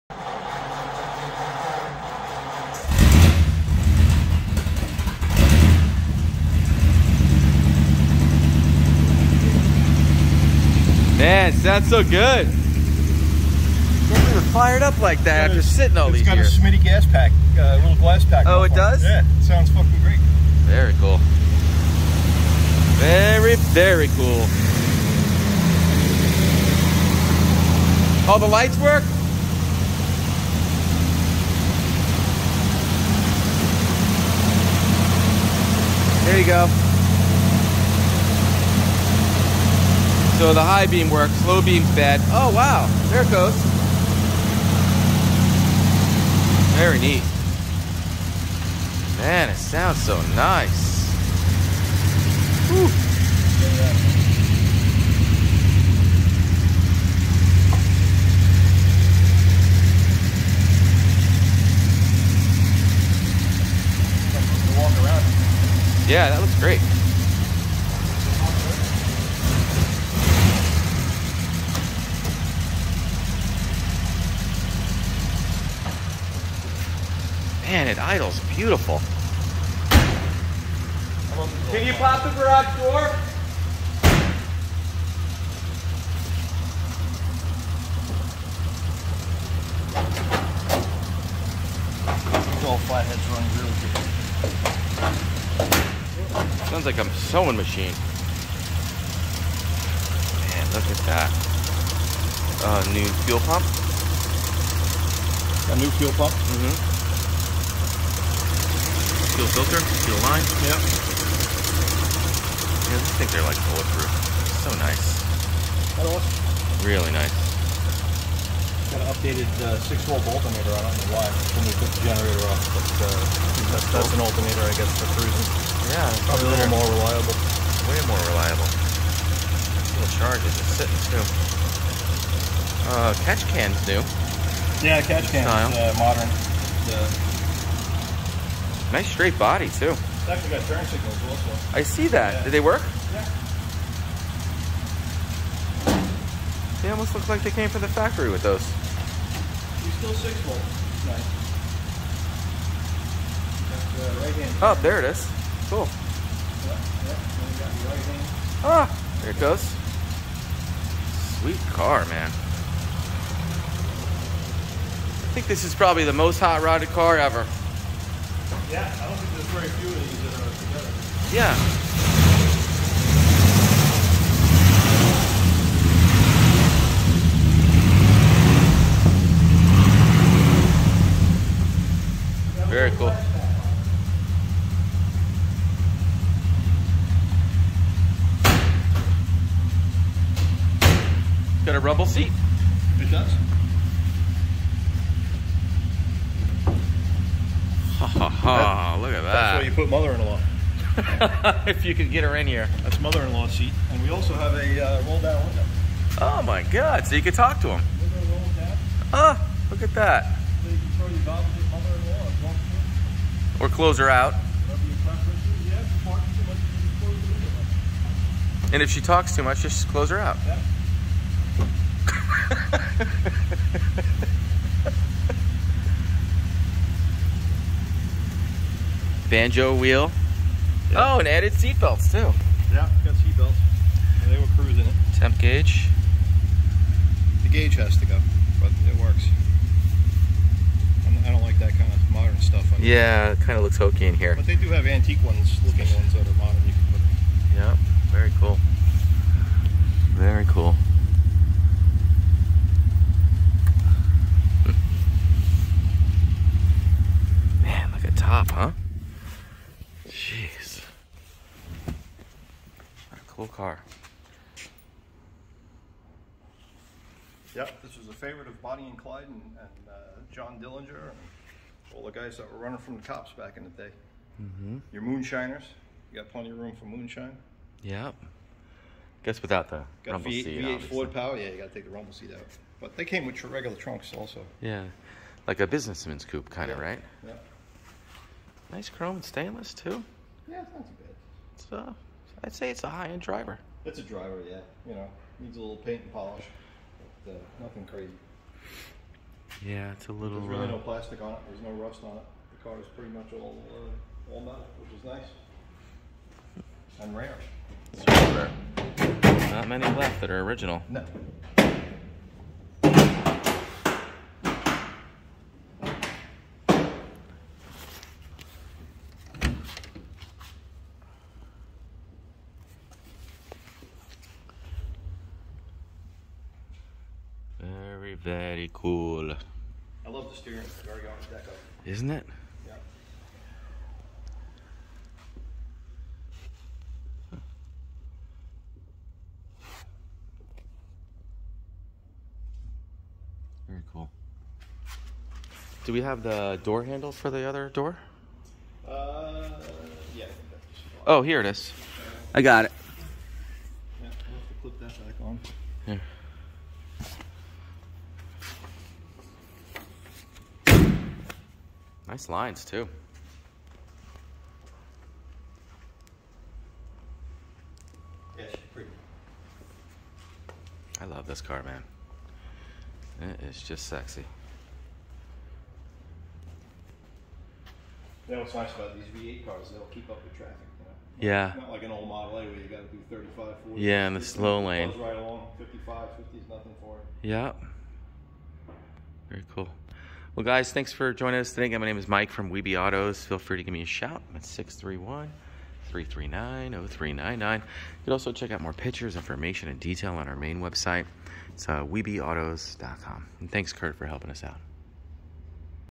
Fantastic, Very Original 1936 Ford 5 Window Coupe For Sale~Flathead Motor~Rumble Seat
1936 Ford 5 Window Coupe For Sale
Fantastic Find in Very Original Condition
Flathead Motor
Manual Transmission
Rumble Seat
Trunk Mounted Spare Tire
Original Center Caps/Trim Rings Wrapped in Wide Whites
Added Turn Signals
Smitty Muffler that Sounds Fantastic
New Fuel Pump, Filter & Lines
Updated 6 Volt Alternator
Modern Temp Gauge
Added Seat Belts
Modern Catch Can
Very Nice Interior with Banjo Steering Wheel
Roll Down Rear Window
Tough to Find in Original Condition, Most of These were Hotroded. Just a Rare Find!
Finished in Lacquer with Some Fish Eyes; Previous Owner Wet Sanded a Few Spots, So that Would Need to be Finished..
Really Runs Fantastic, She's a High-End Driver...No Rust or Issues